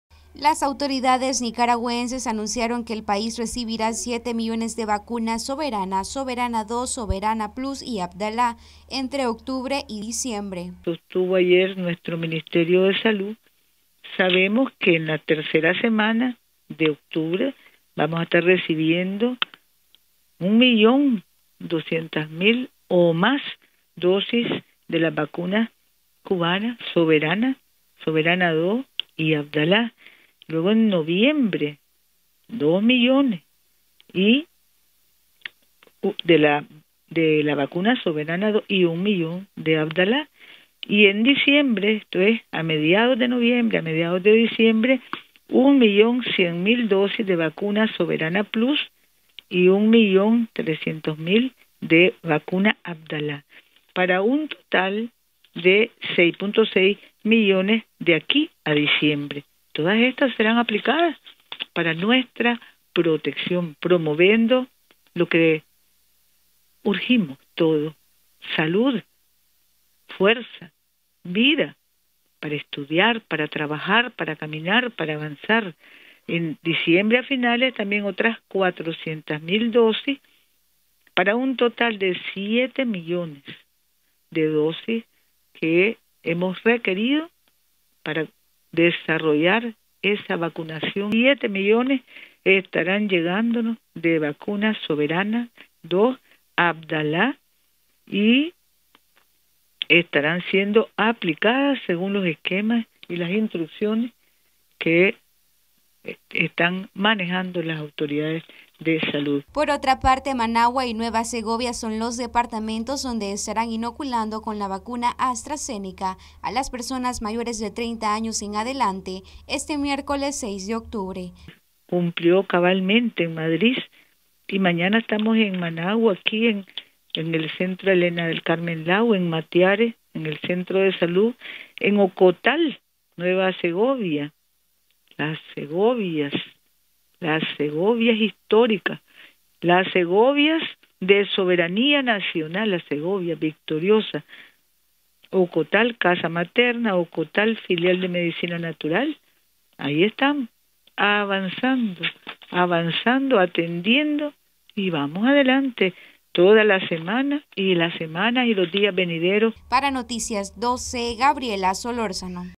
Nicaragua recibirá 7 millones de vacunas Abdala y Soberana entre octubre y diciembre
Noticias12Nicaragua Nicaragua recibirá en los próximos meses 7 millones de vacunas cubanas contra el covid-19, además Managua y Nueva Segovia son los departamentos donde estarán inoculando con la vacuna AstraZeneca